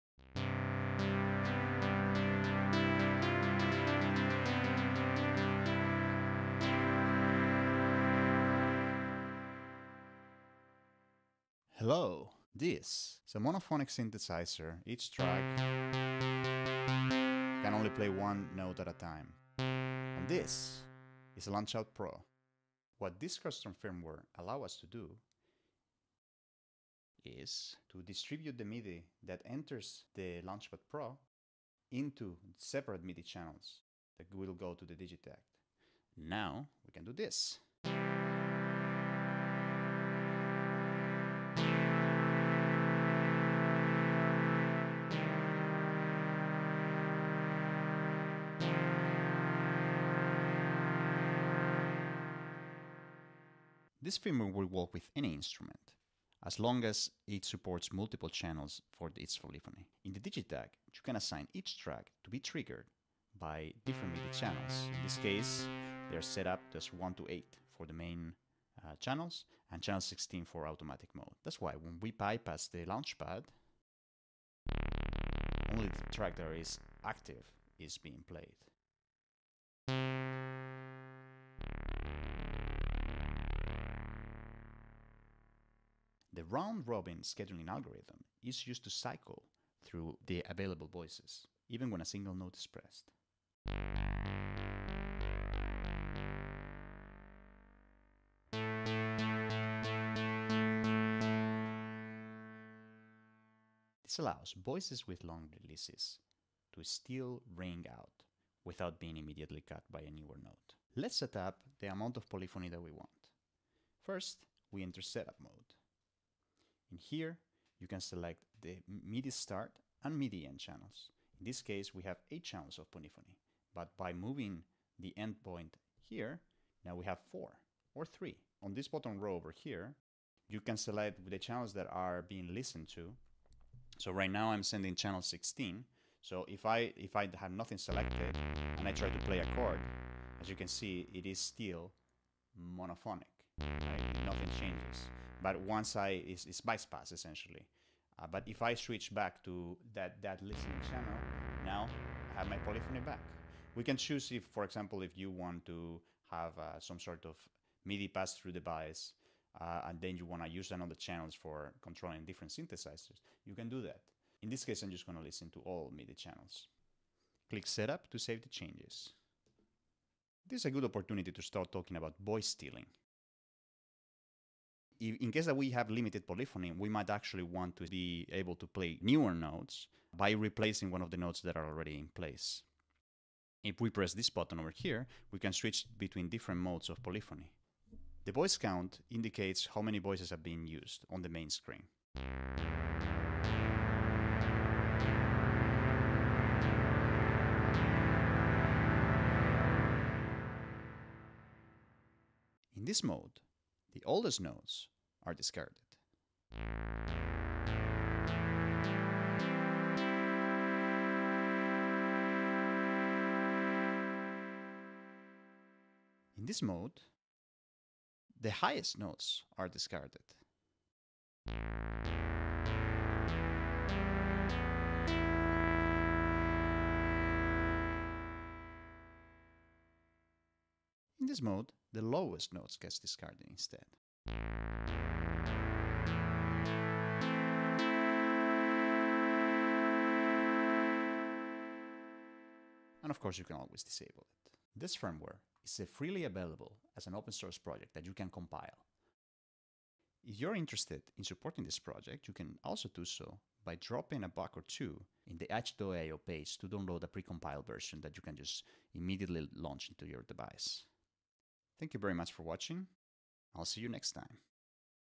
Turn your Digitakt into a polyphonic synthesizer with the Launchpad Pro
I created a custom firmware for the Launchpad Pro Mk.1 that captures MIDI inputs and process them to split them in separate channels. In this demo we show an overview of the features and its application in turning the Elektron Digitakt into a polyphonic synthesizer.

The project is open source and can be downloaded for free
You can also buy the pre-compiled firmware via itch.io to support its development

If you like the work I do, please consider supporting me.

You can also take a look at my or get in touch with me via Mastodon

Cheers!